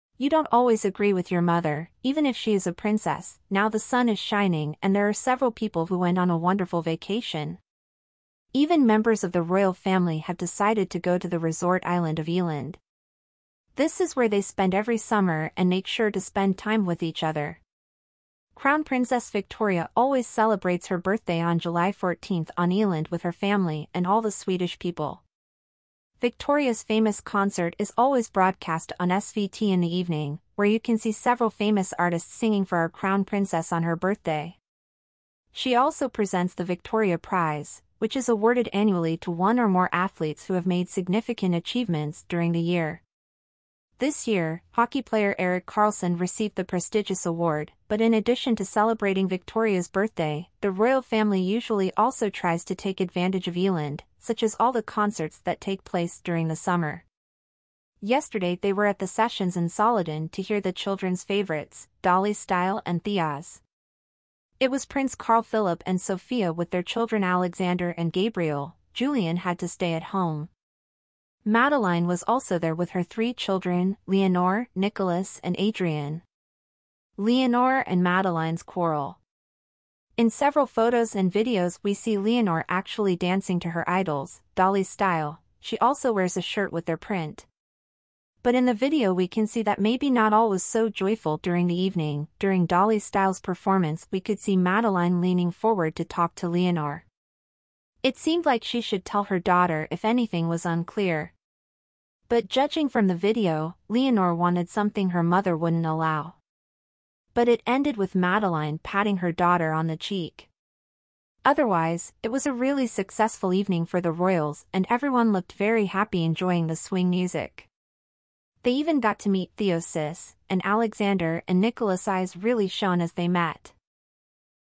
Here's Madeleine telling Leonora there's a fight at the concert.
You don't always agree with your mother, even if she is a princess.Now the sun is shining and there are several people who went on a wonderful vacation. Even members of the royal family have decided to go to the resort island of Eland. This is where they spend every summer and make sure to spend time with each other.